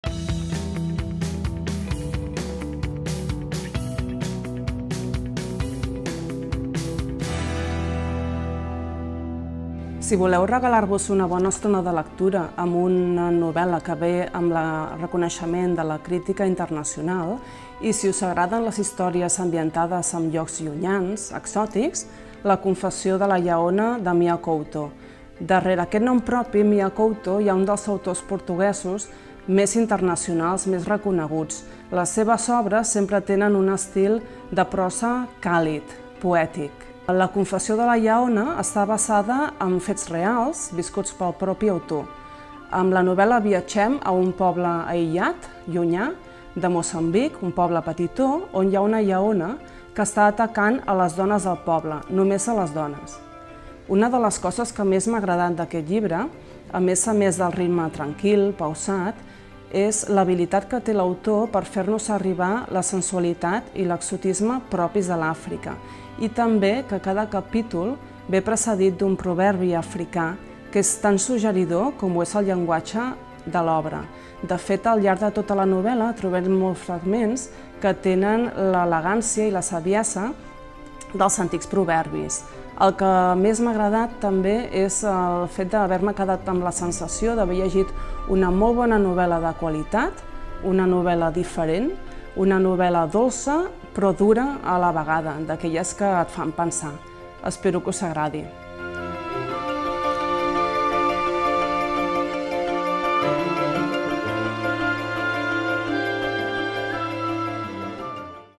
Recomanació. La Confessió de la lleona
La Confessió de la lleona / Mia Couto.
Troba el document al catàleg Aladí / i a l'apartat els bibliotecaris recomanen

Ressenya:
La confessió de la lleona ens transporta a l’enigmàtic Kulumani, un poble aïllat de Moçambic, on les tradicions i creences ancestrals es veuen amenaçades des que una lleona fantasmagòrica ha començat a matar les dones que hi viuen.

Videorecomanació feta per Mar García. Bib. Pare Fidel Fita. Arenys de Mar.